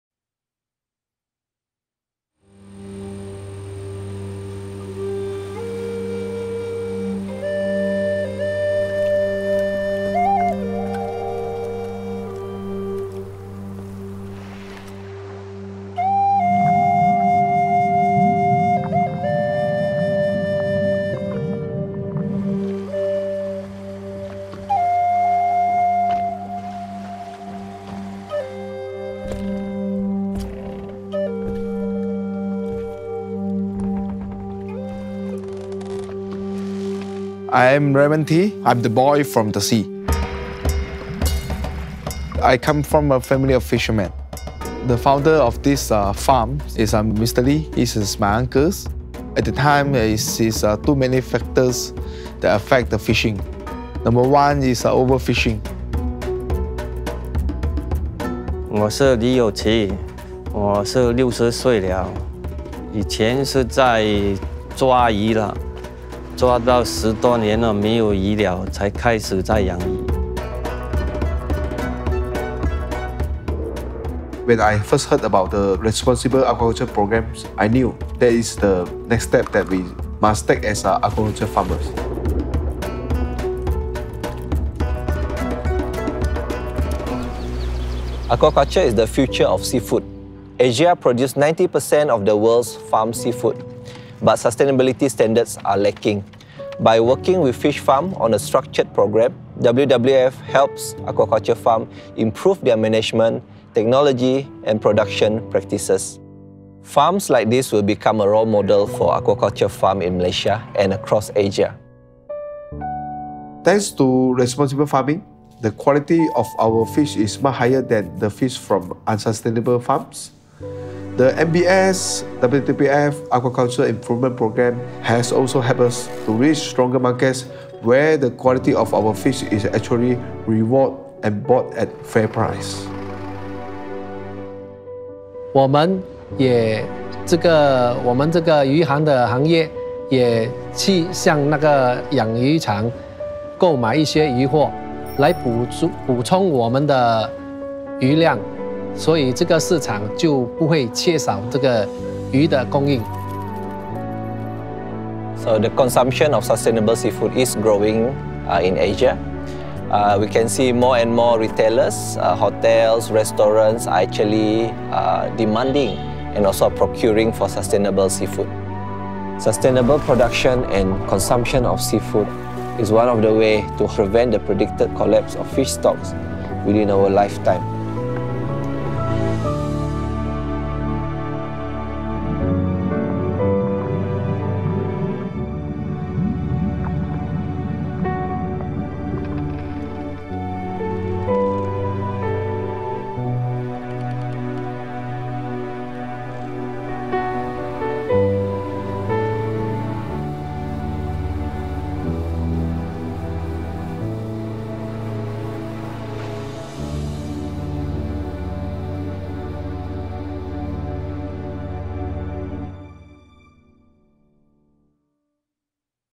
Marina Bay Sands & WWF - Sustainable Seafood Starts with Aquaculture
- Video produced by Global Initiatives -

Almost half of the seafood we eat comes from farms. For fish farming (aquaculture) to hold its promise to meet our future food demands, it will need to demonstrate that it can produce better farmed seafood. Through a partnership, Marina Bay Sands and WWF have jointly developed measurable and achievable goals to improve responsible procurement and transform its supply chain.

Watch the story of aquaculture farmer Raymond Tea and his journey to help supply high-quality sustainable seafood.